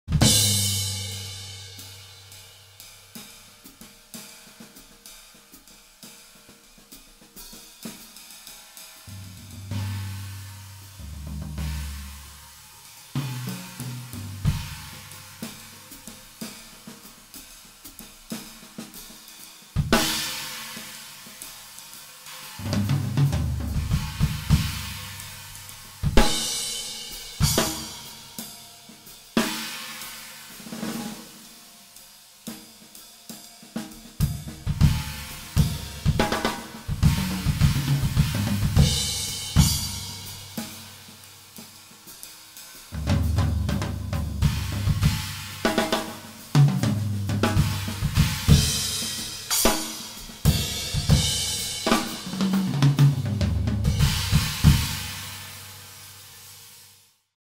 Zildjian Sound Lab Prototype 2020 – #7 Maxx Furian
Maxx Furian con alcuni dei nuovi Prototipi Zildjian in esclusiva per l'Italia.
Set-up:
19" K Sweet Crash Brilliant
10" K Sweet Splash
21" K Custom Staccato Ride
22" K Constantinople Swish - con 6 rivetti
18" K Sweet Crash Brilliant

Registrato con Zoom Q2n-4K